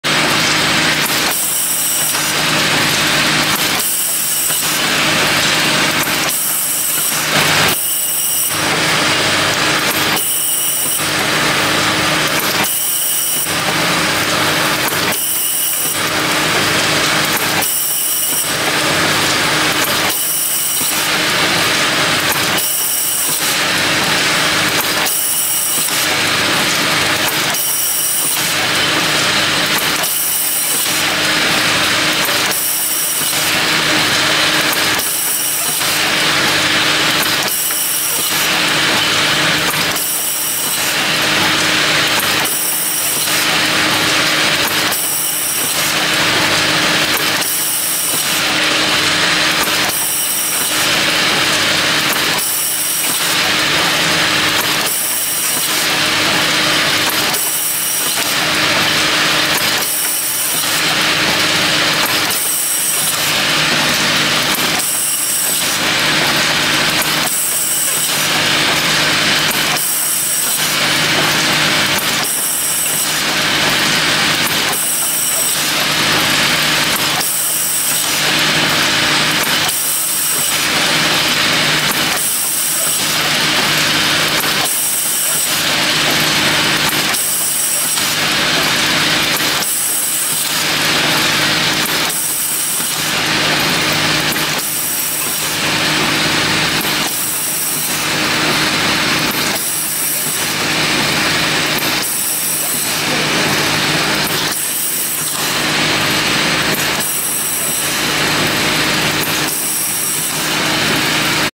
DIY Filling Machine: Build Your Own Automatic Liquid Dispenser
"DIY Filling Machine: Build Your Own Automatic Liquid Dispenser" - Guide your audience through the process of building a simple filling machine using readily available materials, providing step-by-step instructions and highlighting its potential uses.